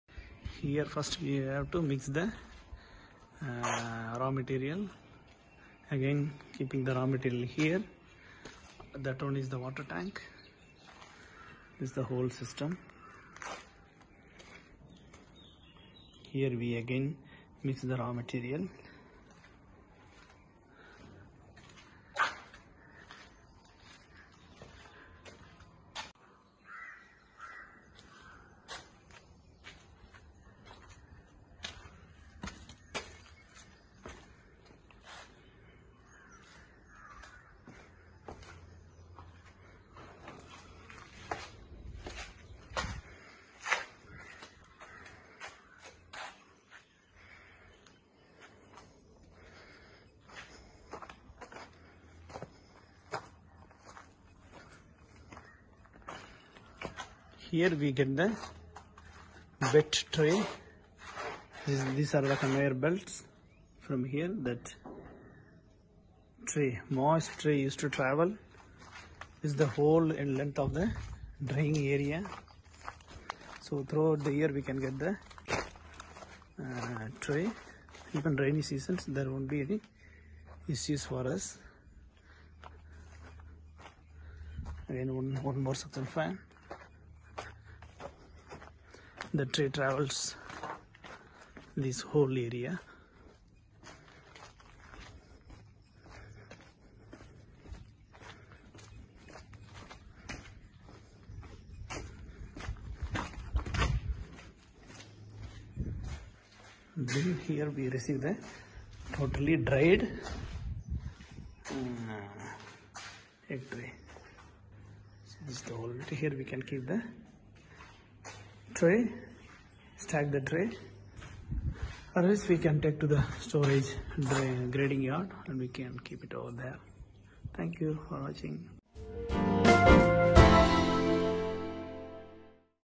Feedback Video in India - Installation of BTF3-4 Egg Tray Production Line
Beston has received feedback video from Indian customers. They installed BTF3-4 egg tray production line with brick drying system successfully!
There is a huge demand for egg trays in India. And Beston Group has cooperated with Indian customers many times. This BTF3-4 egg tray equipment can produce 2000 pieces of paper egg trays per hour. It's a project which uses waste paper as raw material and creates biodegradable pulp egg trays.
If you're interested in this business, no matter which country you're in, Beston can offer one-stop service!